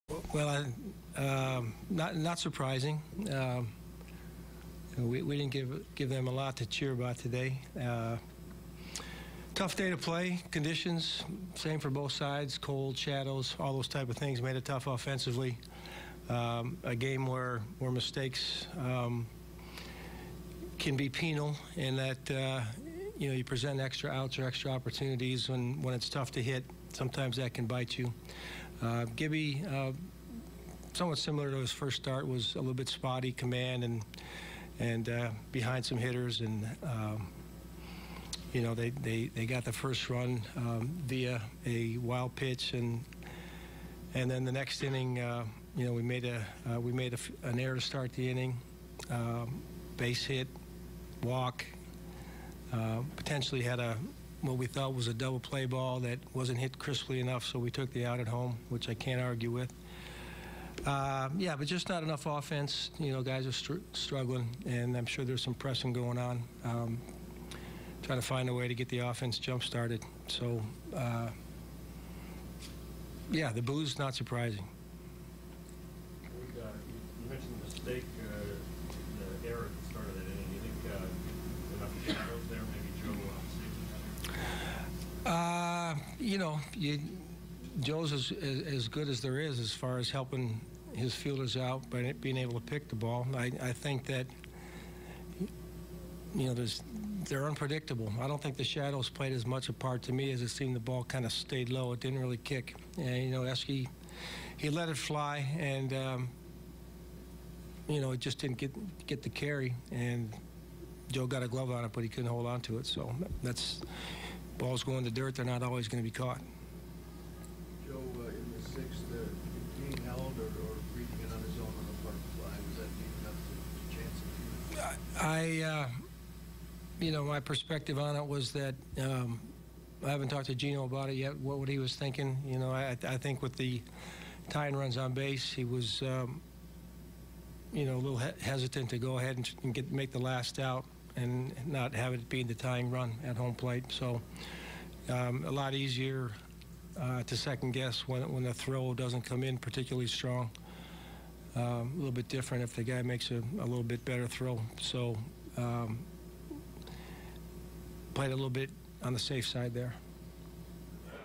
Twins' Molitor on 7th straight loss: 'The boos are not surprising'
Minnesota Twins manager Paul Molitor reacts after home opener loss to Chicago White Sox adds up to 0-7 start